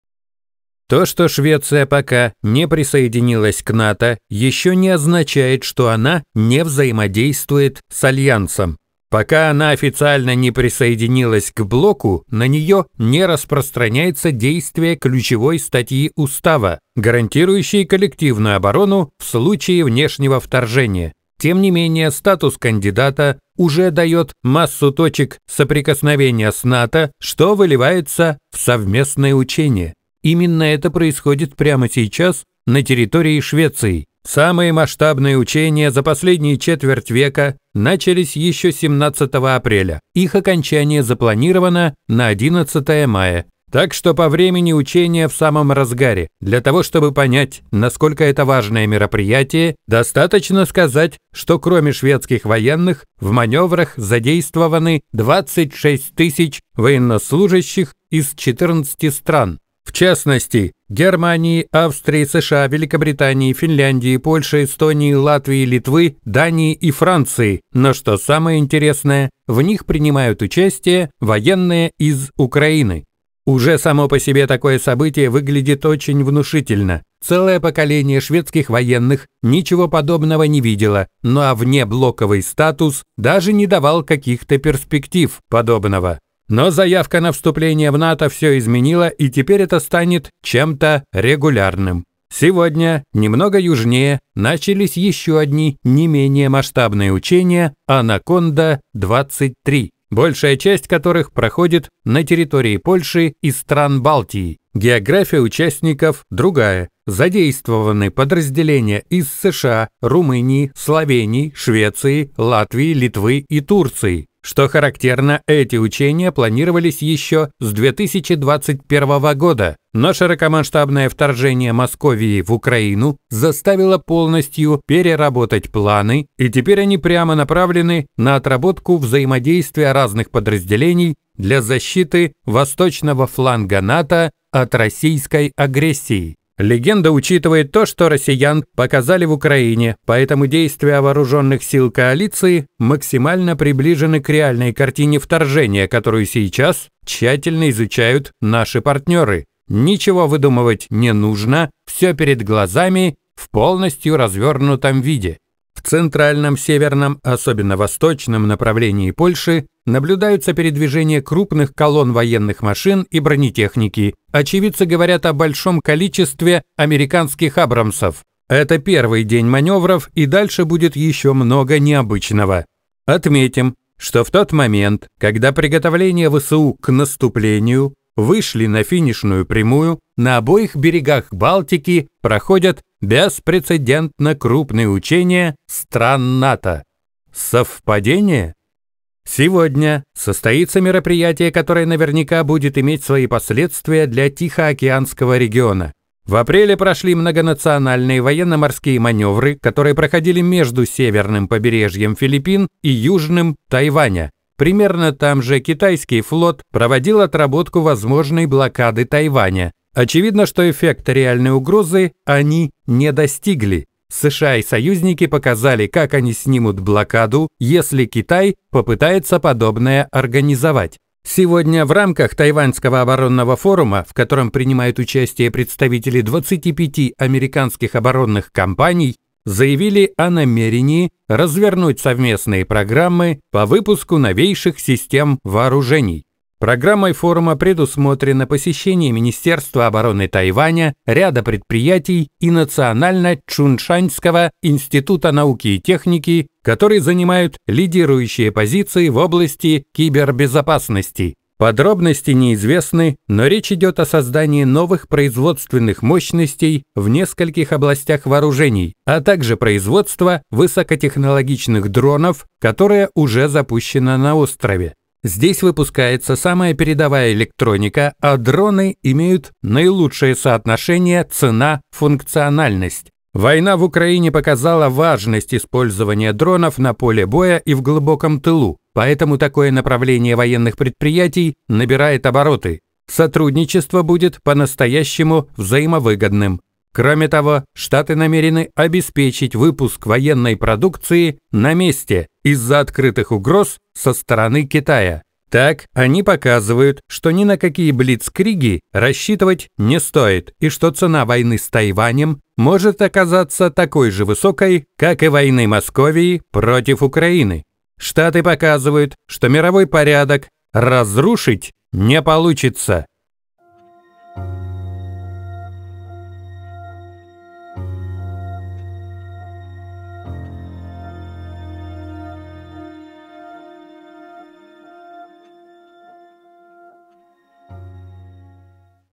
В одни ворота. НАТО доходчиво намекает Москве чем закончится игра в "новый мировой порядок"
Автор: anti-colorados
НАТО отрабатывает "ударный кейс", Тайвань готовится к хай-тек войне